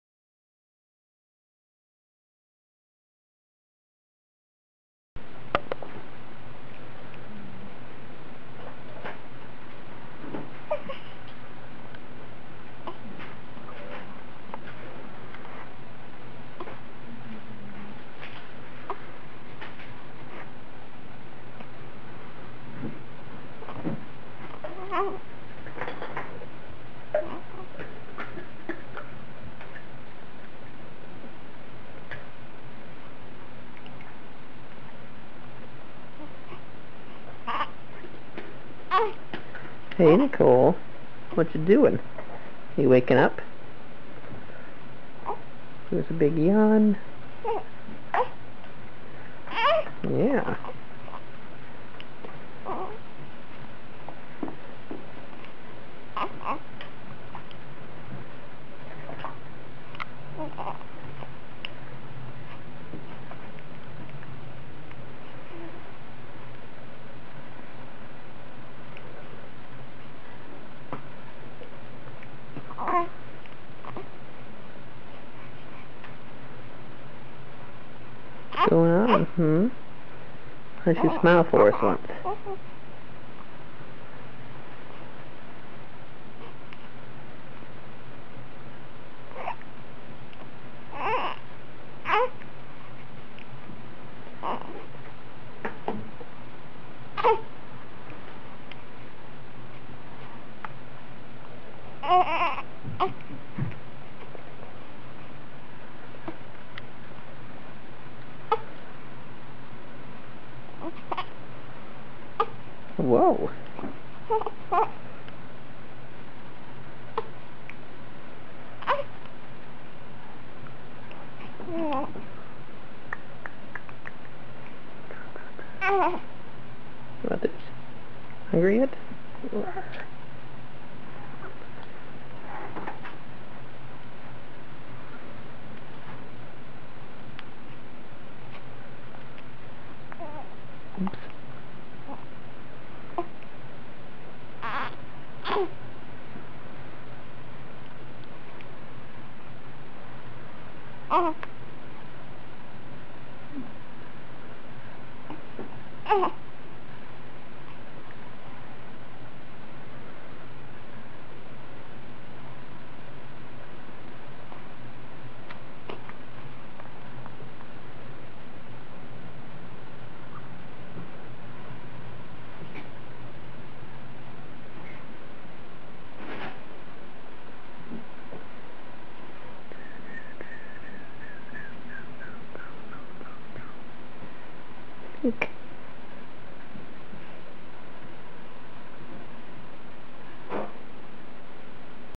Nicole waking up on boppy pillow
Nicole waking up after taking a nap with Daddy, she wakes up slow.